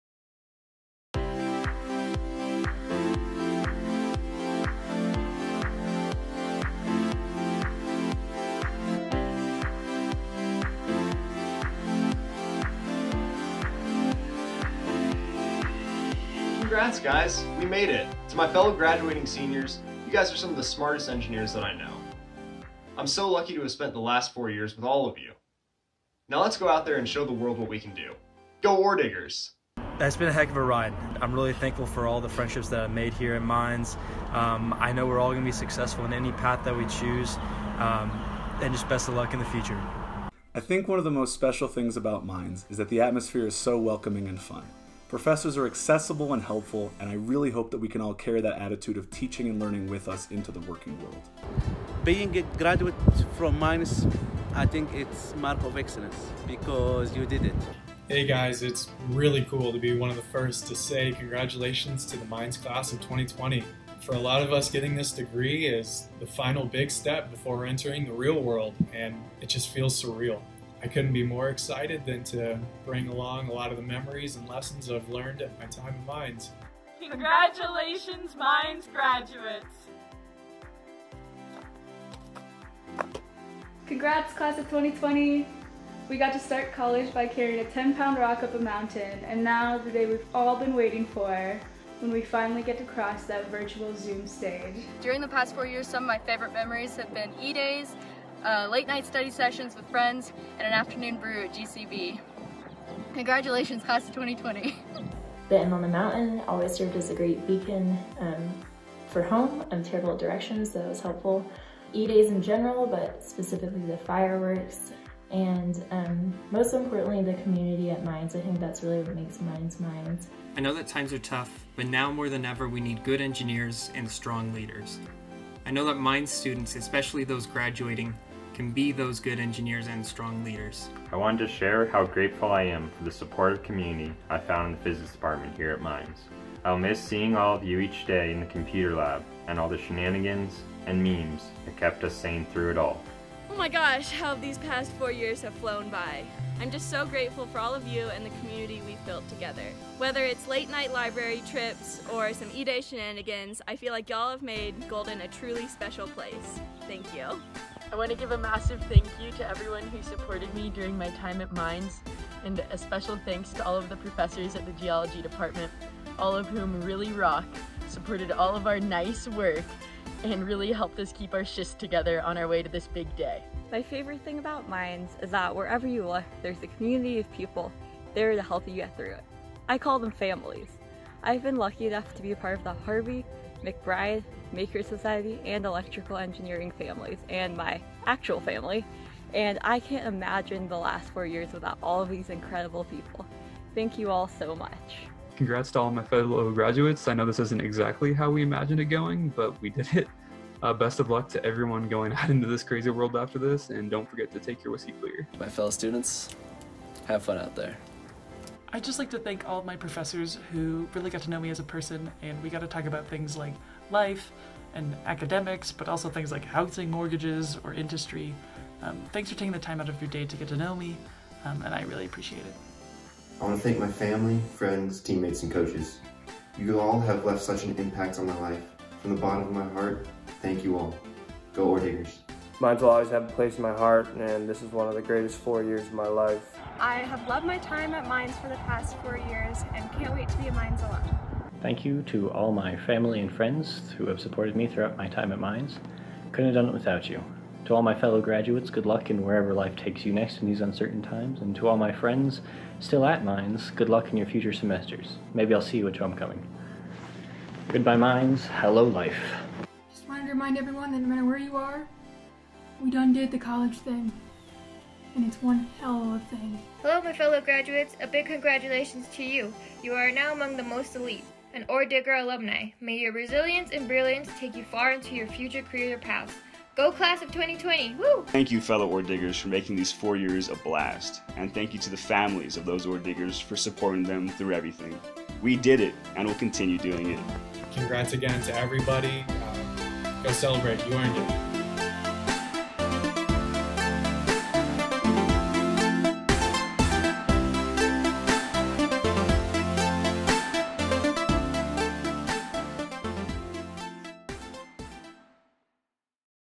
A few words from Mines 2020 graduates
Graduating students share thank-you's, congratulations and favorite Mines memories.
Colorado School of Mines held its virtual ceremonies on May 8, 2020.